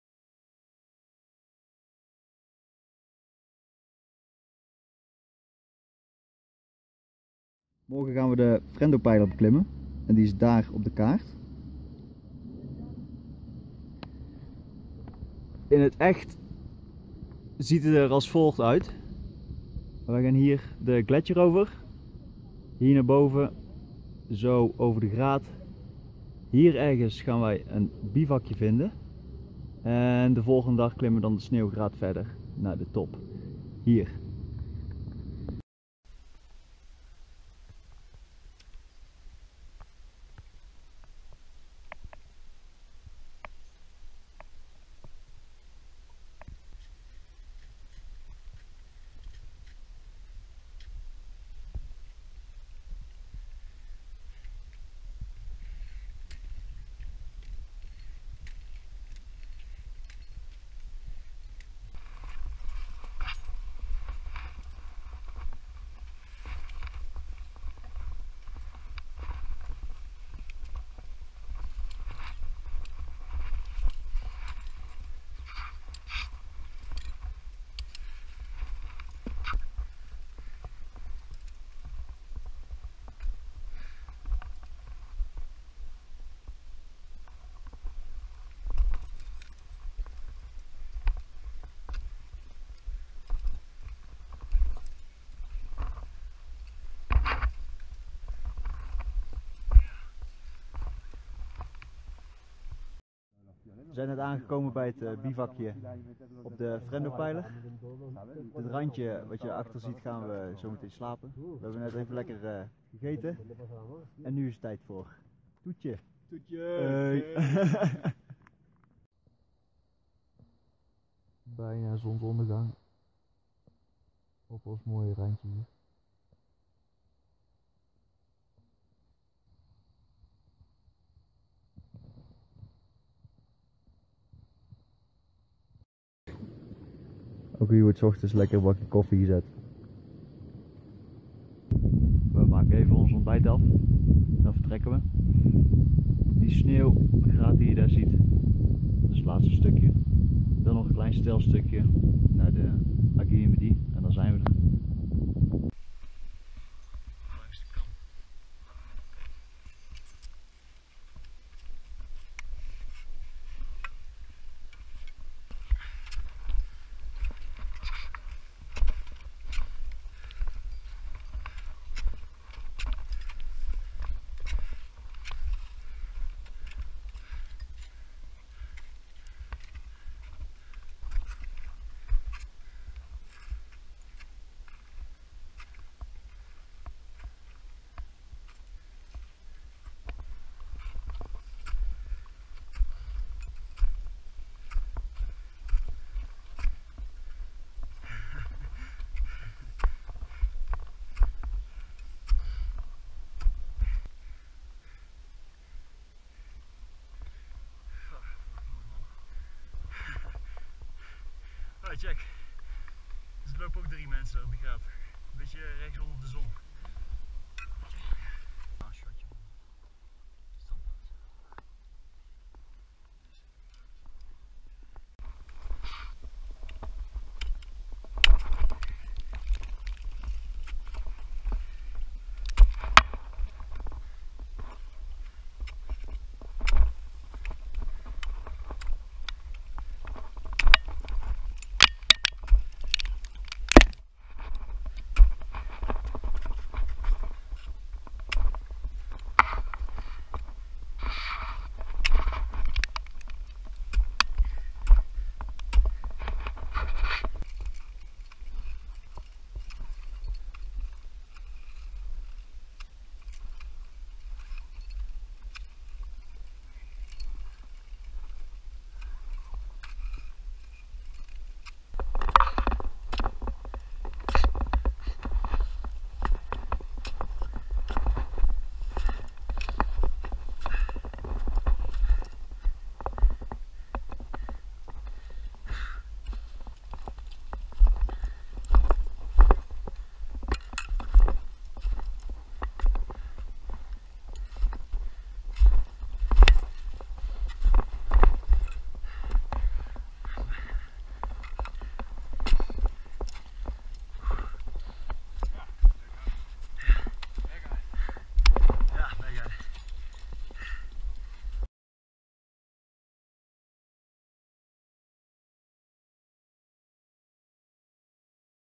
Frendo Spur Aiguille du Midi
Ascending the Frendo Spur on the Aiguille du Midi. It was the first multiday climb for the both of us. We started on Juli 18 and went up rock and mixed terrain to reach our bivouac at around 3250 mtr. Especially the lower part of the rock just after the start had snow left in it. The rest untill the bivouac was as good as clean. We spent the next morning on the last snowridge and ice wall before reaching the summit. In early morning the snowridge was in perfect condition. The steep ice wall was as well but with a lot of others parties in the route it had to be shared and falling ice couldn't always be avoided. In al we took a little longer than the time indicated in the guide book but this was also due to our heavy backpacks for out planned bivouac. Overall it was a very pleasurable climb.

Climbers:
Jarron Middelhuis & Rik van Odenhoven

Footage & editing:
Rik van Odenhoven